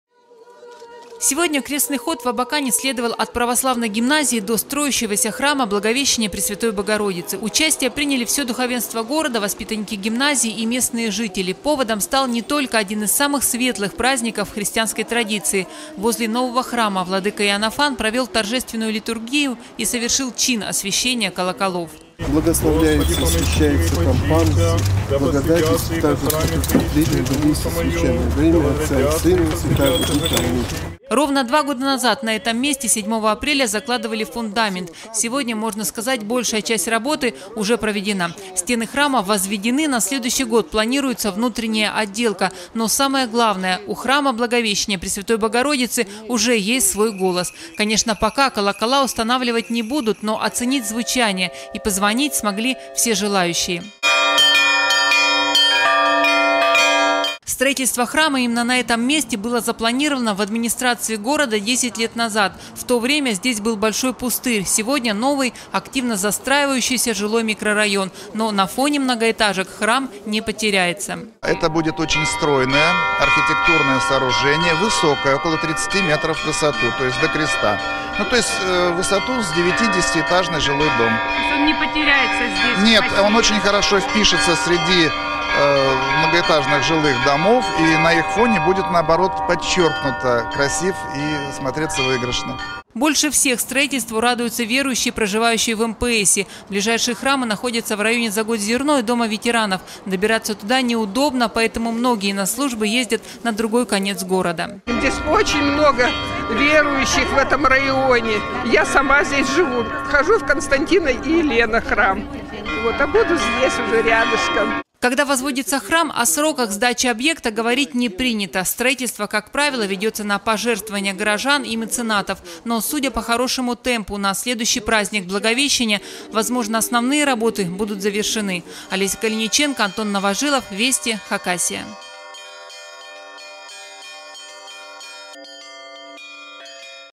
Православные христиане отмечают великий праздник Благовещение Пресвятой Богородицы 07.04.2016
Для верующих Абакана этот день стал особенным. В районе МПС провели Крестный ход и совершили чин освещения колоколов для строящегося храма.